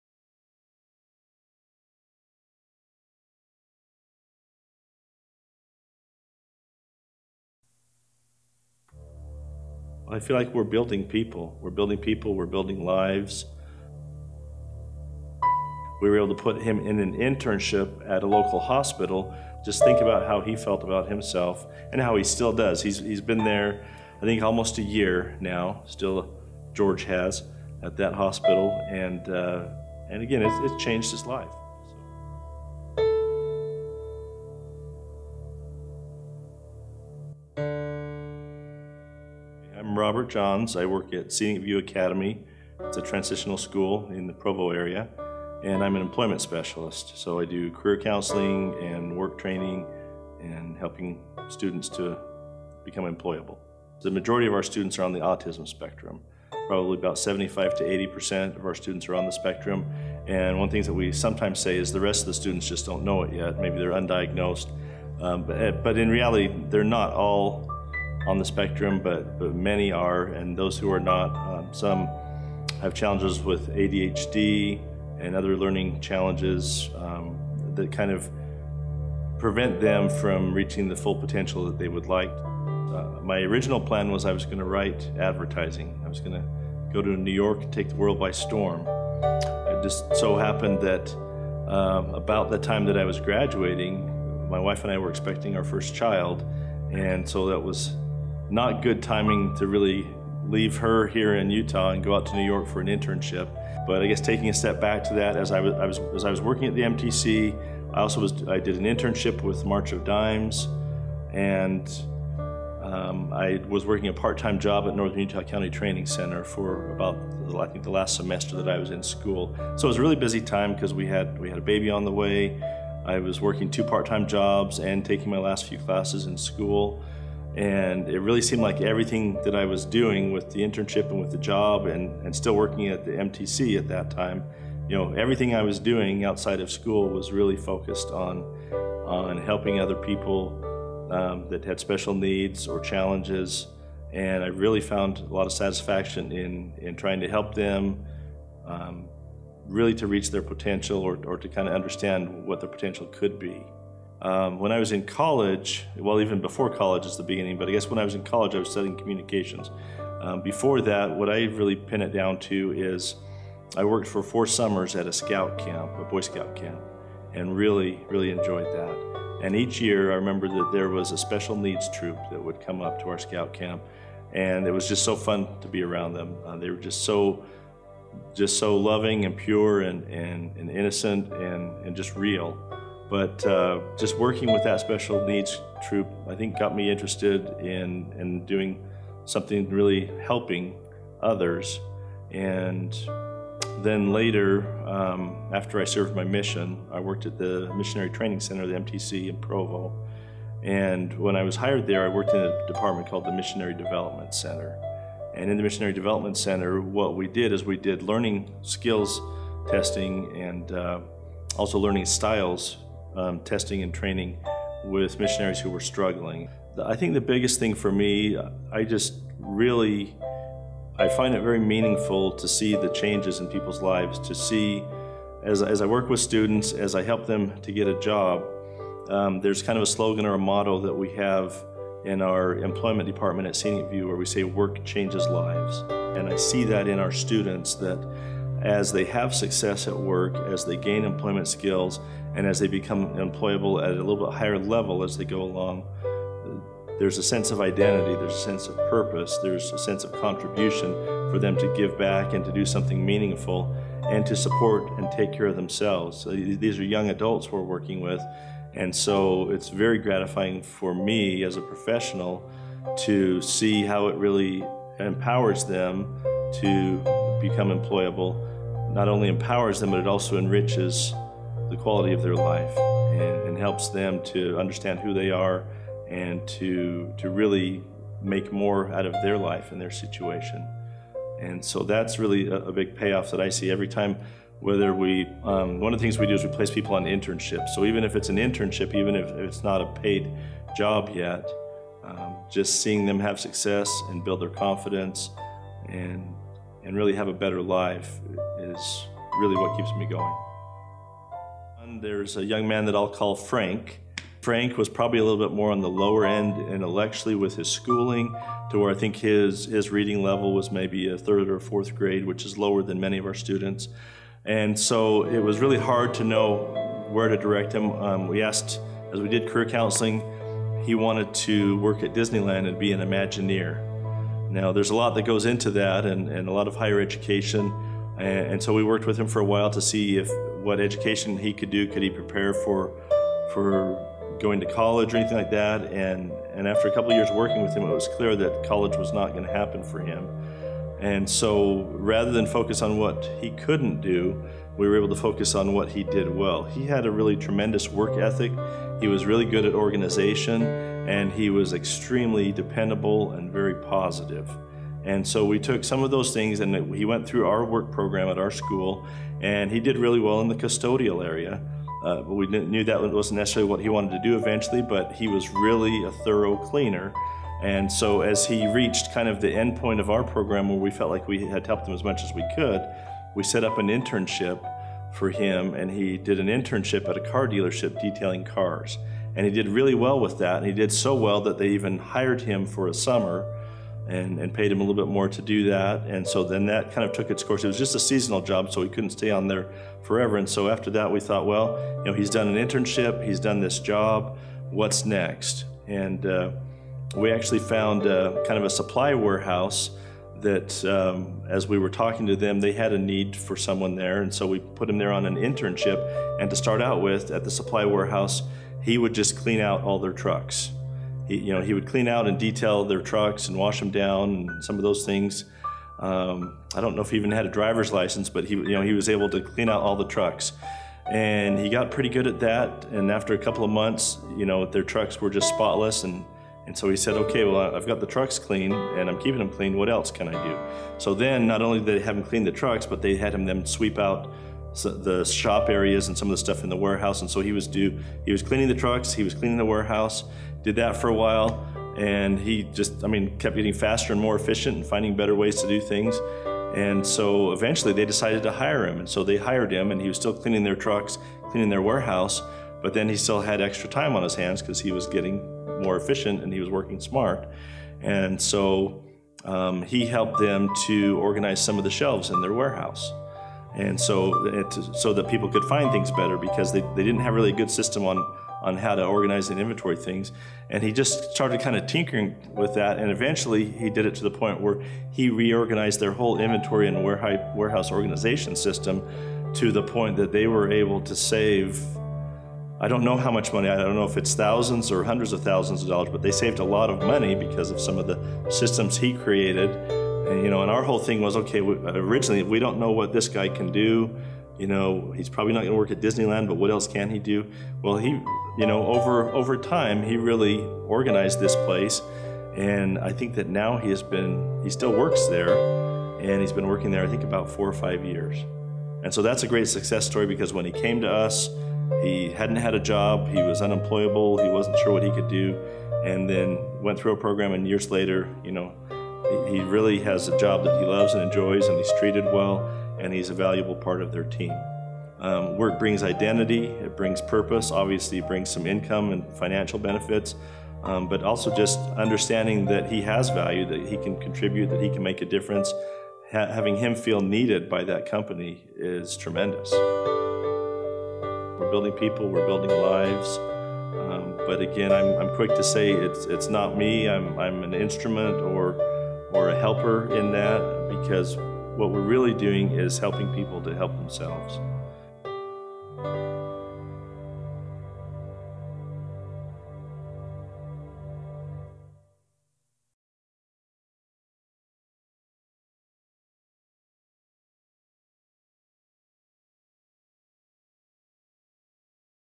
Documenting Public Service
Inspiring Stories. Extraordinary People.

Robert is an employment specialist and career counselor at ScenicView Academy, a school for adults with autism and learning disabilities in Provo, UT.

Filming, editing and production by Matt Imbler.
Music composed and performed by Matt Imbler.